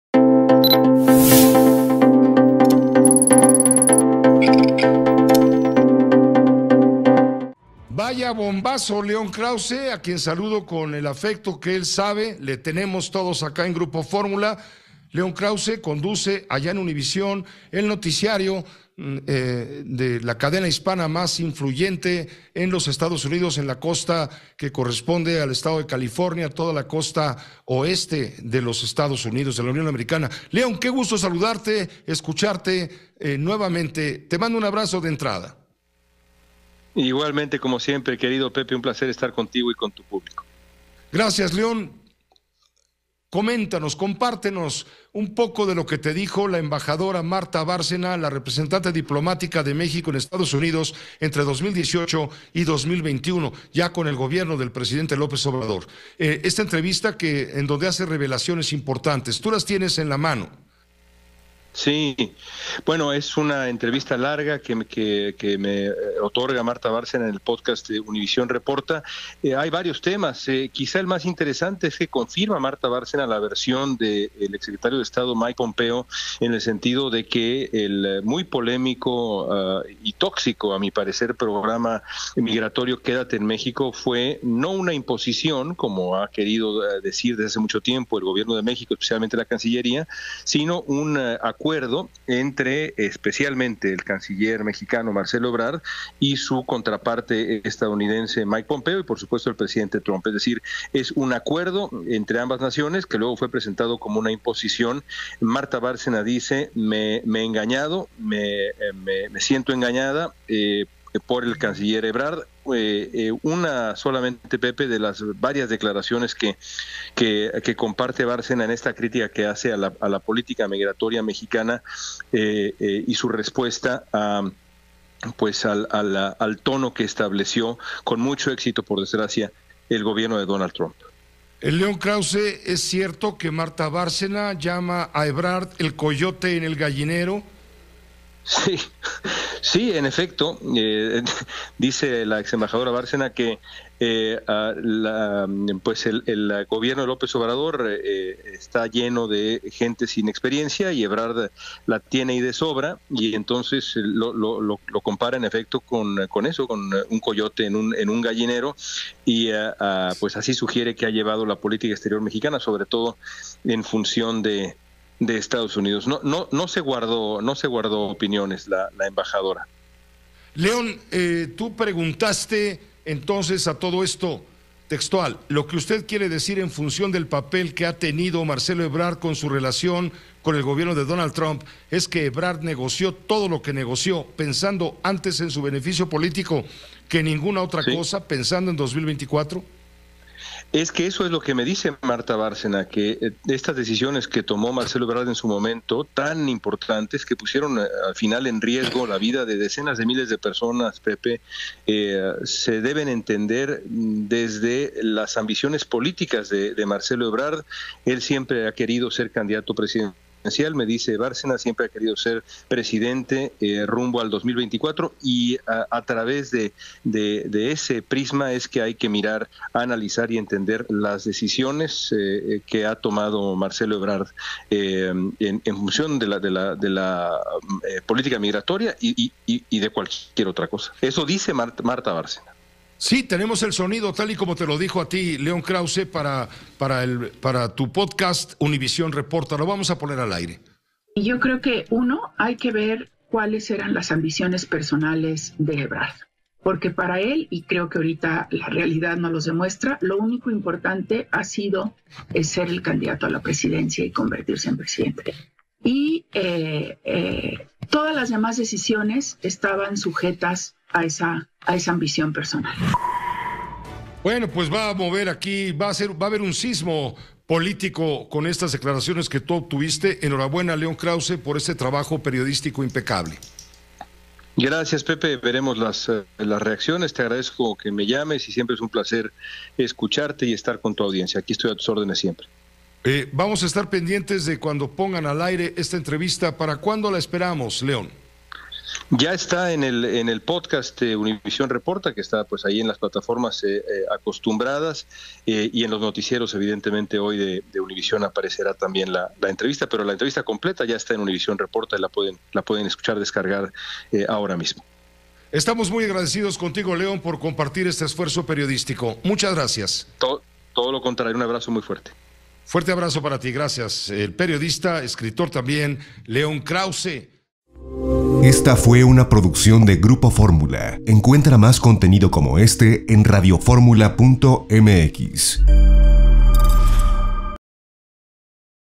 Denuncia acuerdos migratorios, entre Ebrard y Pompeo: Martha Bárcena
La exembajadora de México en Estados Unidos, Martha Bárcena, confirmó que el canciller Marcelo Ebrard aceptó el acuerdo que daría paso al polémico programa «Quédate en México» y le pidió mantenerlo en secreto. Dicha información ya había sido revelada por Mike Pompeo, Secretario de Estado en el gobierno de Donald Trump.